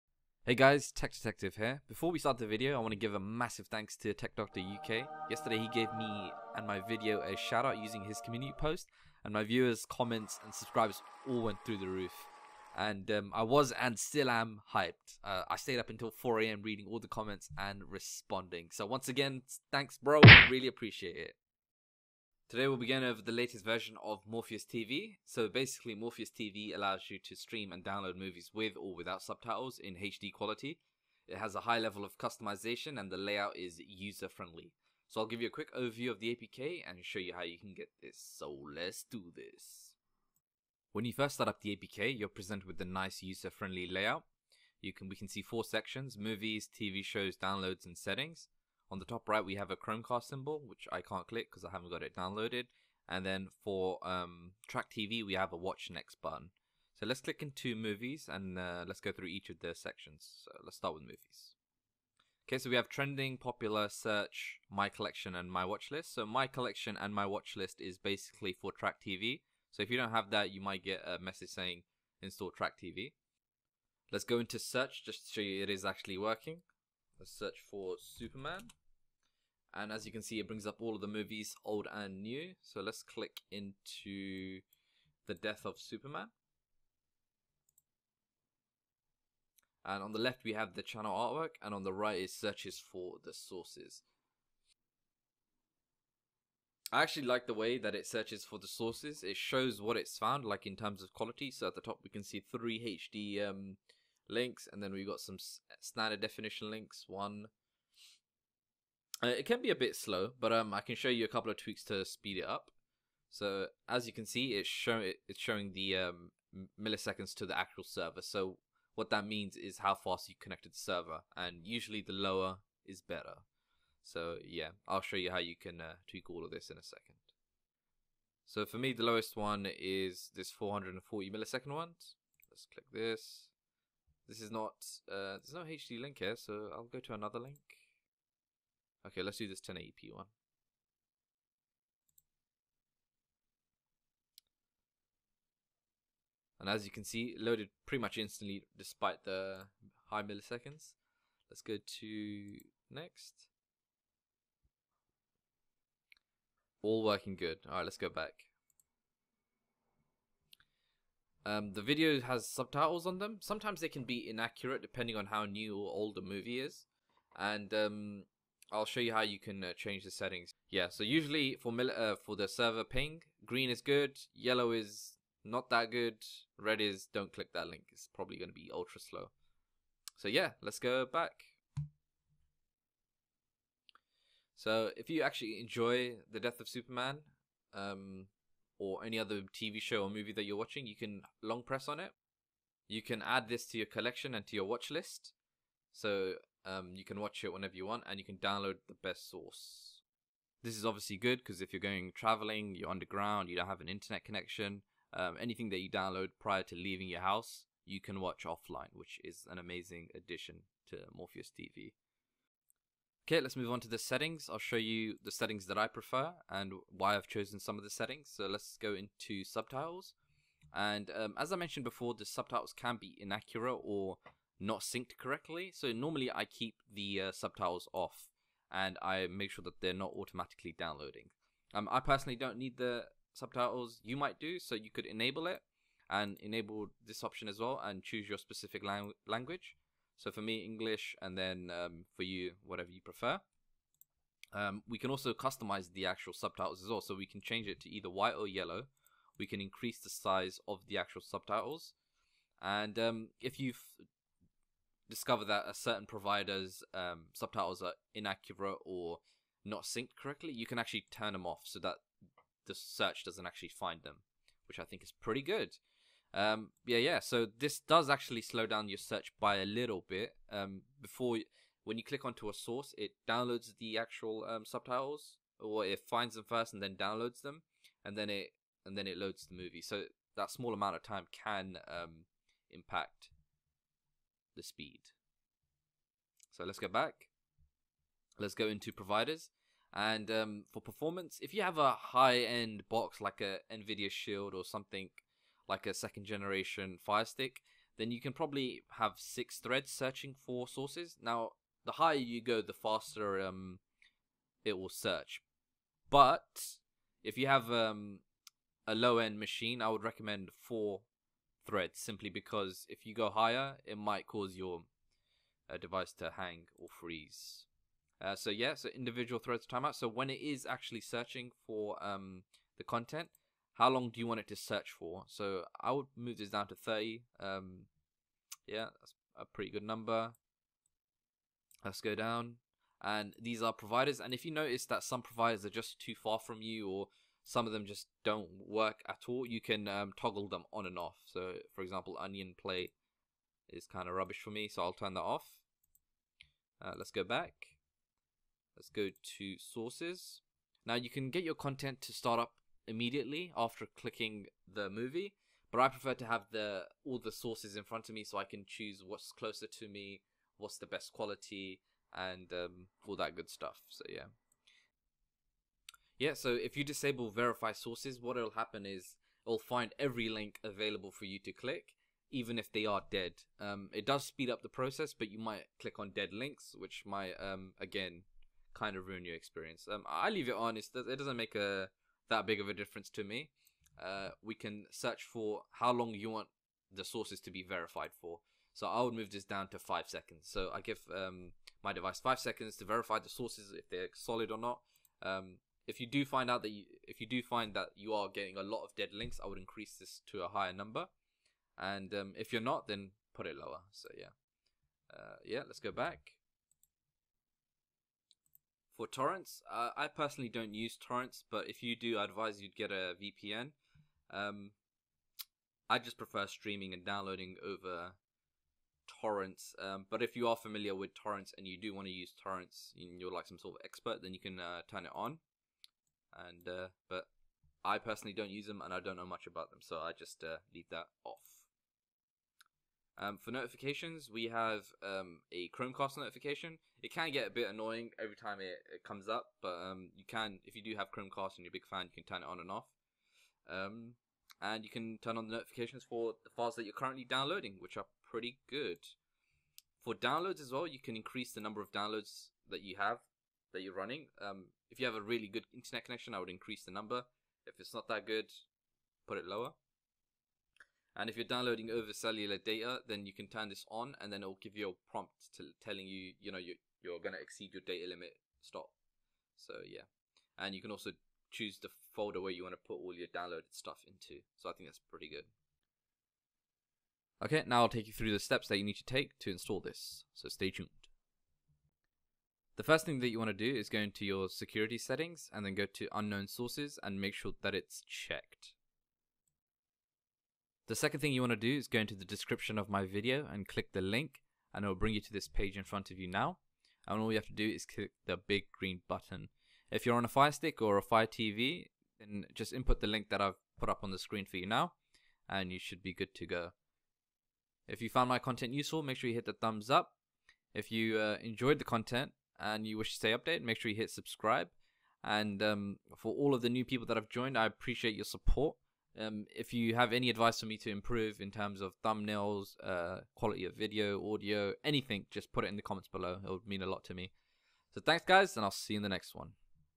MorpheusTV Download Free Movies & TV Shows No Kodi! No Ads!
Want to watch free movies and tv shows? Want to be able to download them and watch them offline?

in this video i'll be showing you the latest version of morpheus tv and the best settings to use. I'll also be showing you how you can get this free apk to stream, and download movies in HD. No Kodi, No Ads!